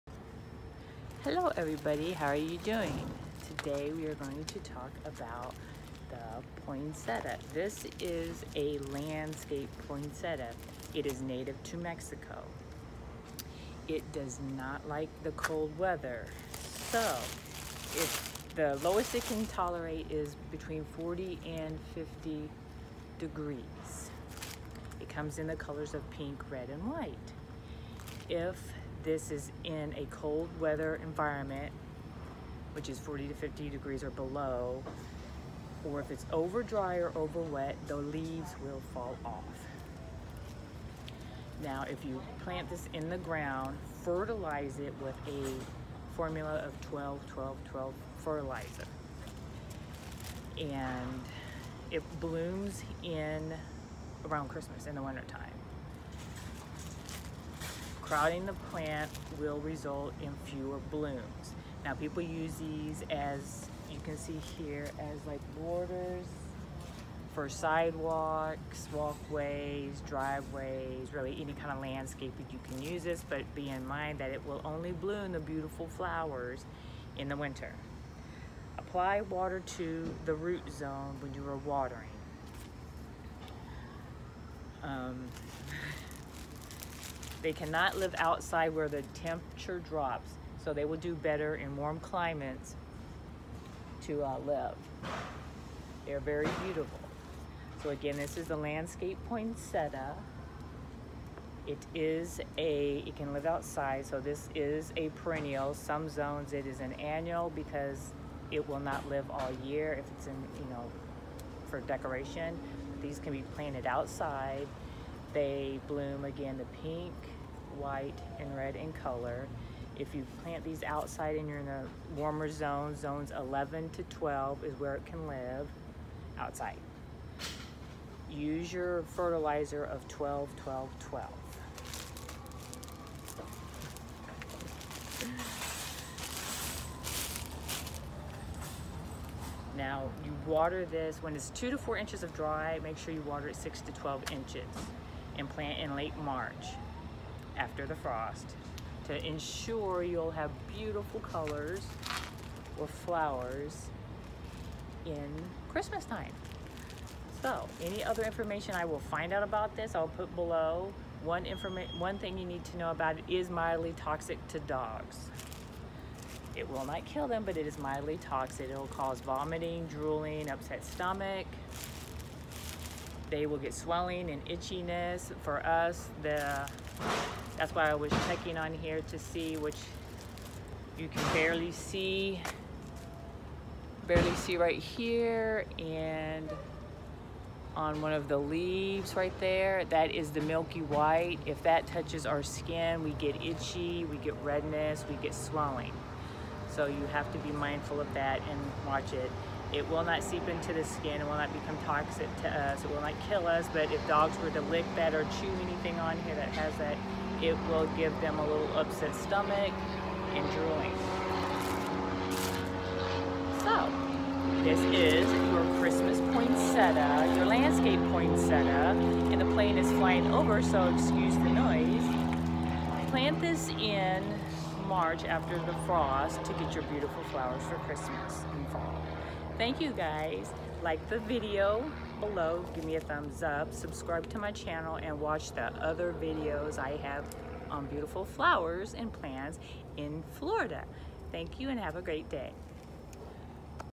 How to Grow Beautiful Landscape Poinsettias
Hey great to see you here.
My name is Becky and I am going to tell you a little bit about the Poinsettia. You must be here because you want to know something on them right. I can help you with that. Lets get to it.

The Landscape Poinsettia will be found in zone 11 - 12 In its native regions in Mexico growing outside. The Poinsettia blooms beautiful colors of pink, red, white by nature ONLY in, you guessed it winter. Yet winter in Mexico is still warm as if it is summer. Poinsettias can grow 8 - 10 feet tall & 12 - 18 feet wide and can handle temperatures as low as 40 degrees. They can not live outside in areas where the temp falls below 45 degrees Poinsettias can not tolerate the frost. And do not plant in full sun but keep the soil consistently damp. not soaking wet. Take note here that if it is exposed to cold drafts or excess warmth the soil is over dry or to wet WILL result in leaf drop. Also crowding them WILL result in few blooms. And we all love those big red eye catching blooms don't we? When you plant the Poinsettia make sure to use starter fertilizer to give extra boost with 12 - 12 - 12. When in a pot indoors make sure to water 6 - 12 inches from the base of the plant. Not until it is 2 - 4 inches dry. When it comes to pruning them if you're are lucky enough to live in a warmer zone. Here is a hint do it after the last frost in late March to get more blooms in the winter.

** In the early 70's this plant was marred with being mildly toxic to dogs and cats. The milky white substance found in the Poinsettia is difer penoid esters & saponin like detergents. The symptoms will be vomiting, drooling, diarrhea. If you handle them keep in mind that if you get the milky white sap on your skin you can develop skin irritation, redness, swelling, itchiness. Medical treatment is rarely needed unless signs are severe. But you'll be glad to also know that they were tested by Ohio State University and they proved they are NOT toxic to humans or pets.